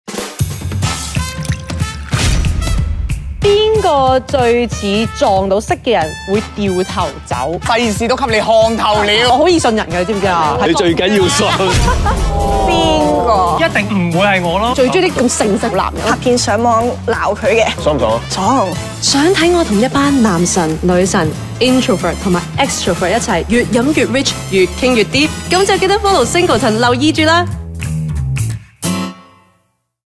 The Singleton - Ring of Curiosity 系列
相聚於酒桌，總會越飲越Rich，越傾越Deep。今次The Singleton 推出全新卡牌遊戲「Ring of Curiosity」，並邀請了氣氛擔當阿正 @903ahjeng，連同兩組圈內舊友新知，一邊細味The Singleton 12年，一邊試玩遊戲，把酒一場，刷新印象。

「Introvert 大戰Extrovert 篇」率先請來黃德斌 @kennywong8384、簡慕華 @kmw_r 和程人富 @chingyanfu，在酒桌上展開Rich Talk，打破I人和E人的刻版印象。

另一集「男神女神篇」則邀請到栢天男 @mr.adampak、Asha @ashaetc和余德丞 @yuduck，透過辛辣遊戲問題，發掘男女神鮮為人知一面。

誰是小氣之最？誰又是外貌協會成員？密切留意我們的IG和The Singleton Hong Kong YouTube Channel，兩集Spicy 十足的「Ring of Curiosity」足本遊戲內容，Coming Soon！

This year, The Singleton launches a brand-new impression card game ‘Ring of Curiosity’, inviting DJ Ah Jeng @903ahjeng, along with her old pals and new friends, to savour the delightful Singleton 12-Year-Old while playing the game.

In the soon-to-be-launched ‘Introverts VS Extroverts’ episode, we have invited Kenny Wong @kennywong8384, Rachel Kan @kmw_r and Ching Yan Fu @chingyanfu to join Ah Jeng in the game, overturning impressions about introverts and extroverts.

The second episode ‘Prince Charming & Goddess’ features Adam Pak @mr.adampak, Asha @ashaetc and Dickson Yu @yuduck, revealing the unknown sides of the prince charmings and goddesses.